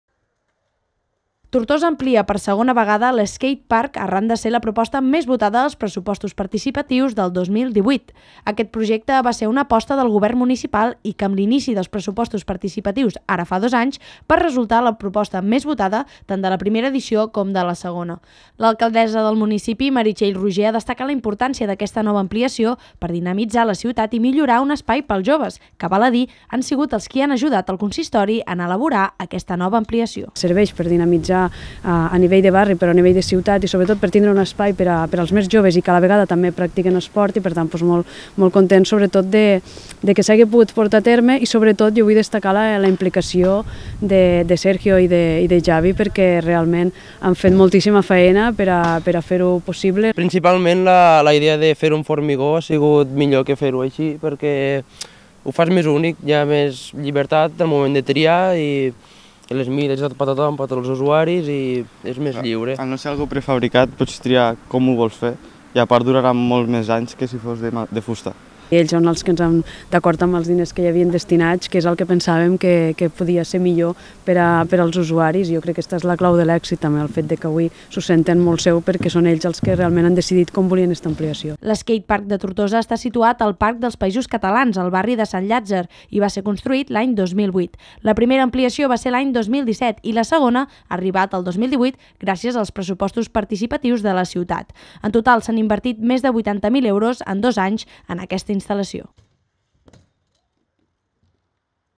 Nova ampliació al Skatepark de Tortosa
Finalitzada la nova ampliació de l'skatepark de Tortosa. En total s'han invertit més de 80.000 euros en dos anys en aquesta instal·lació, ubicada al barri de Sant Llàtzer. Aquest projecte va ser la proposta que va rebre més vots en l'edició dels Pressupostos Participatius tant de l'any 2018 com del 2017.